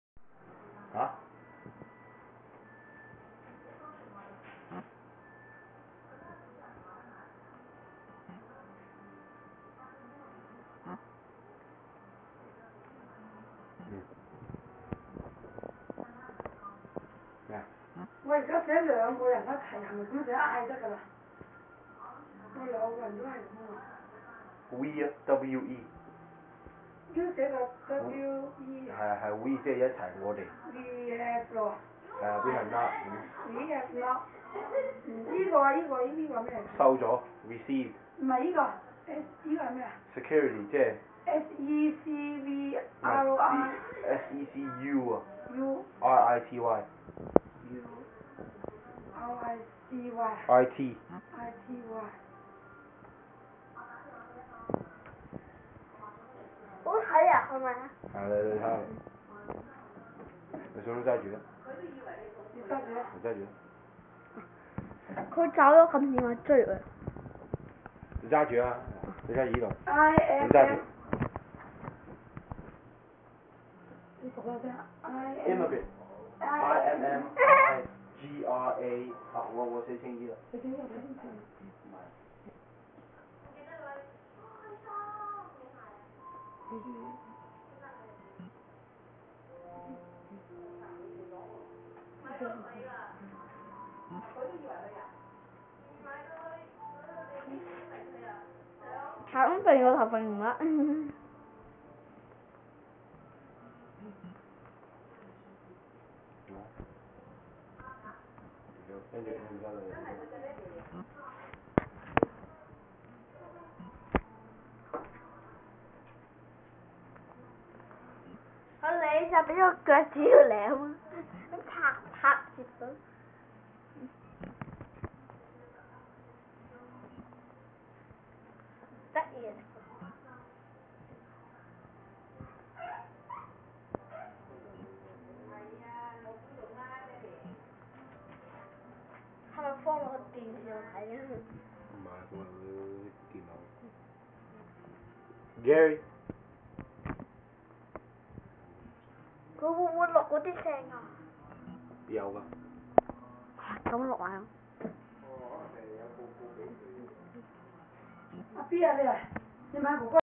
Gary - chihuaha and daschund mix o.o
This is my puppy Gary..Chihuahua and a wiener dog...hows that work?